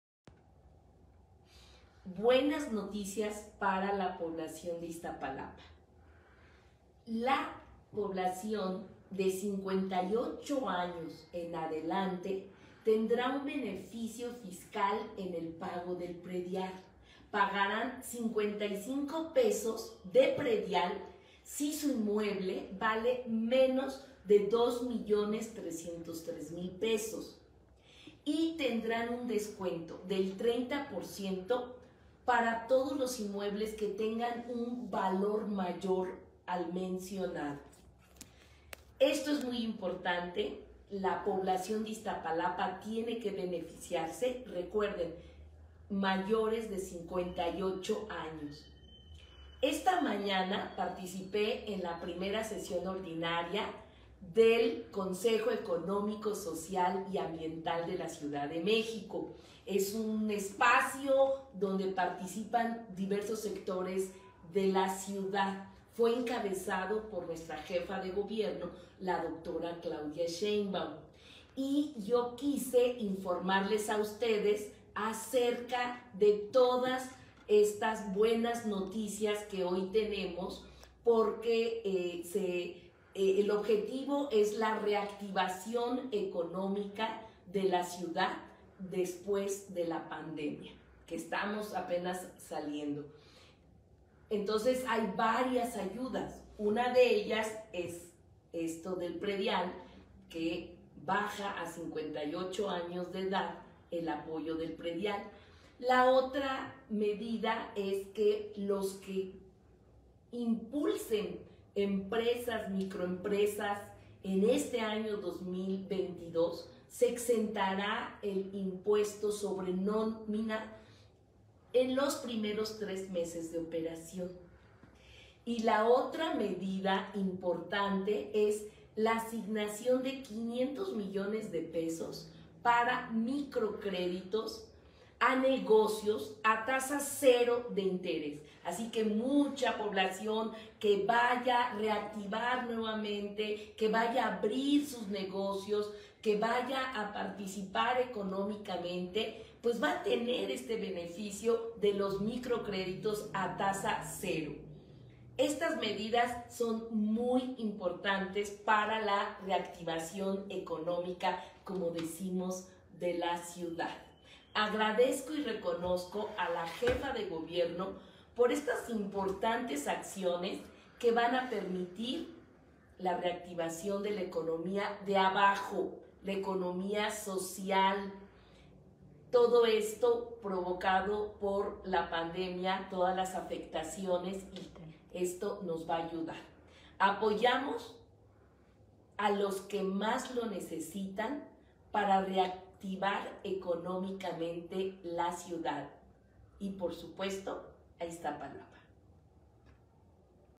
BUENAS NOTICIAS PARA IZTAPALAPA
PERSONAS DE 58 AÑOS PAGARÁN $55 PESOS SI SU INMUEBLE VALE MENOS DE 2 MILLONES 303 MIL PESOS